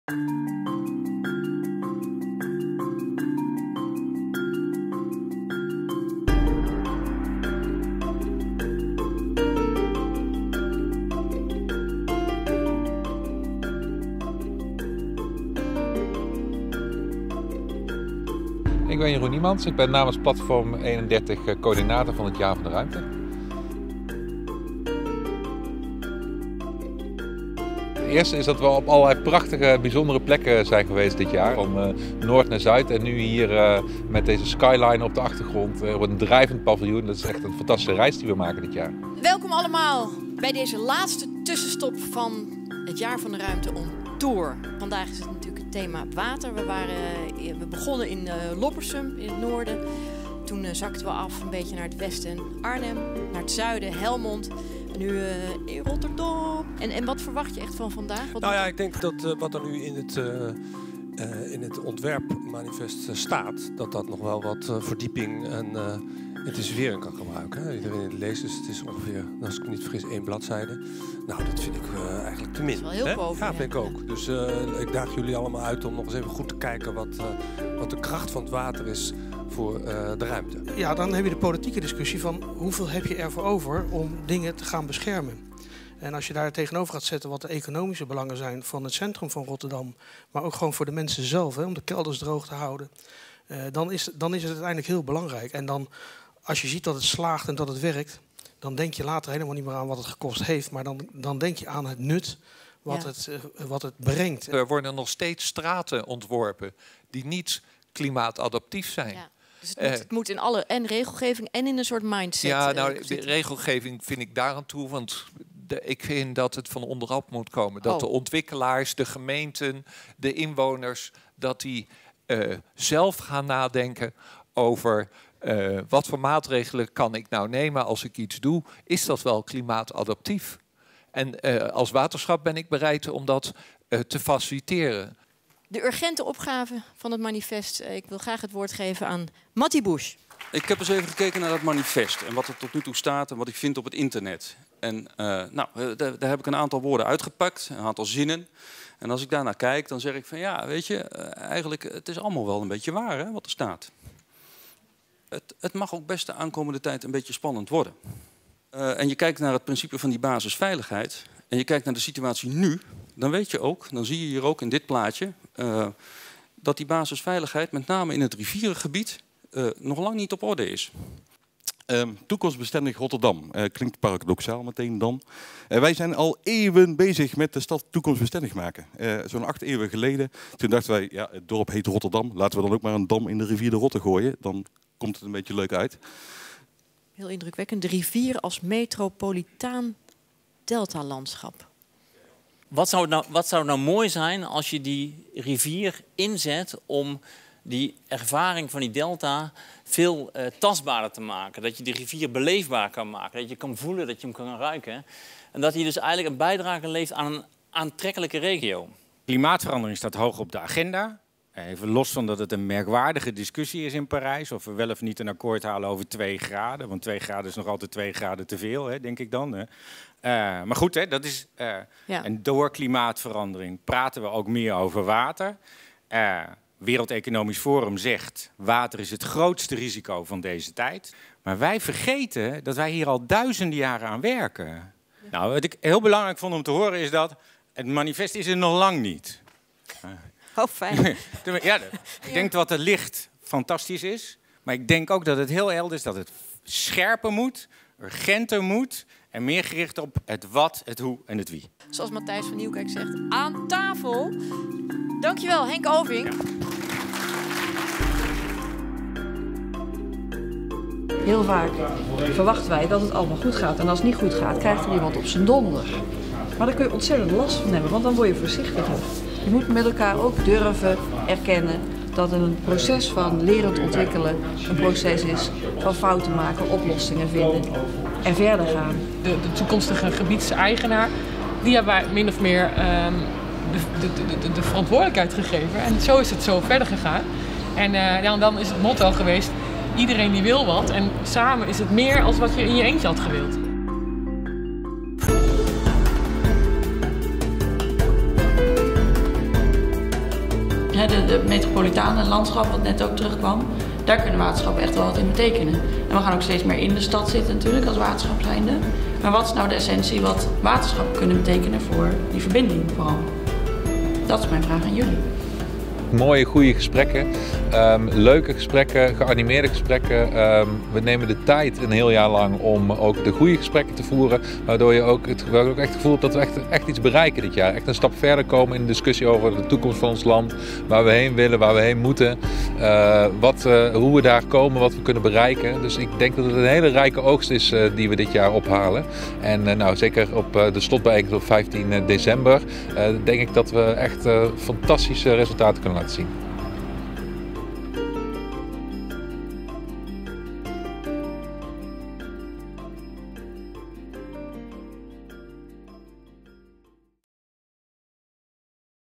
Jaar van de Ruimte on tour #4 - Rotterdam
12 november 2015 @ Drijvend Paviljoen, Rotterdam
Rotterdamse Delta
Tijdens de bijeenkomst op 12 november staat de verbinding van water- en ruimtelijke opgaven centraal. Veel mensen zijn zich nauwelijks bewust van de rol van het water in hun leefomgeving. Ook beleidsmatig zijn ‘water’ en ‘ruimte’ nog steeds twee werelden. Lees op wijmakennederland.nl meer over deze opgave. In de Rotterdamse Delta komen veel opgaven samen.